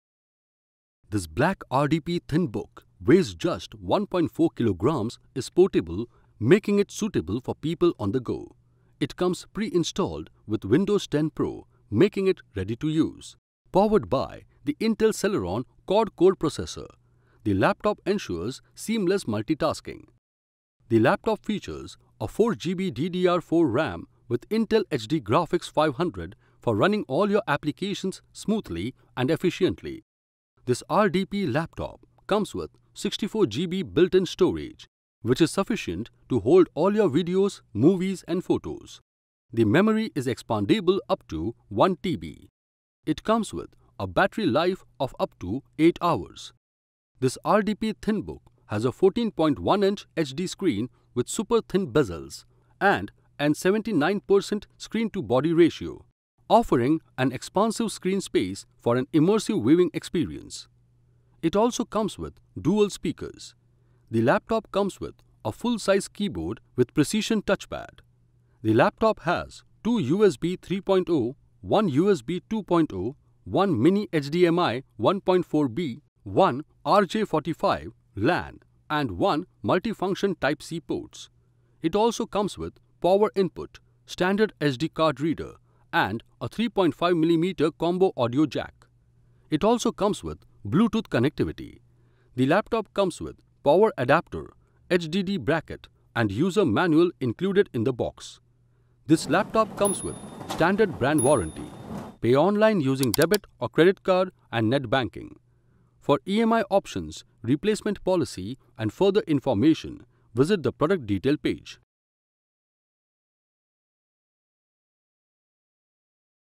Laptop l Laptop under 30000
Intel Celeron Quad core Processor N3450,up to 2..2 GHz, 4GB RAM, 64 GB Built-In storage (HDD/SSD slot, support up to 1TB) Pre- loaded with Windows 10 Pro OS,Thin & Light Wight Design (Just 22mm & 1.4kg), 8 Hours of Battery Beckup for Uninterrupted Day Long Activities,Dualspeaker,web camera,Mic, Bluetooth 4.0Type-c Multifunction port,full- size keyboard, Precision Touchpad Durable and compact Build